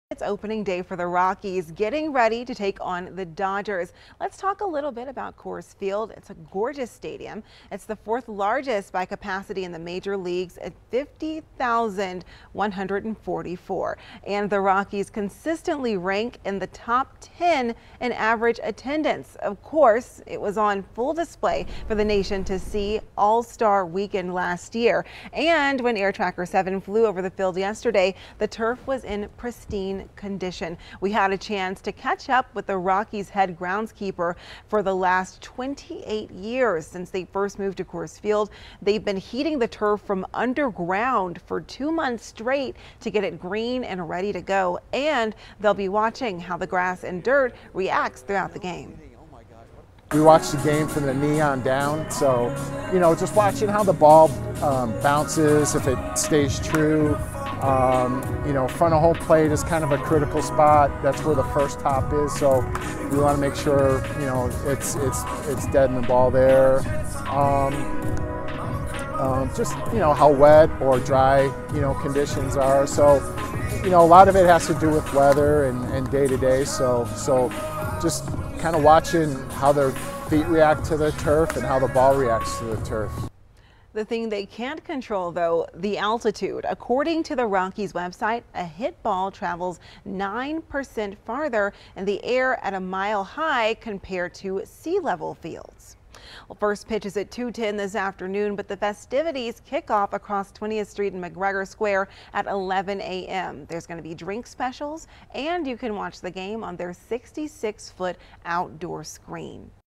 Coors Field and opening day: Fun facts and getting ready for opening day
Staff at Coors Field has been hard at work getting the field ready for opening day.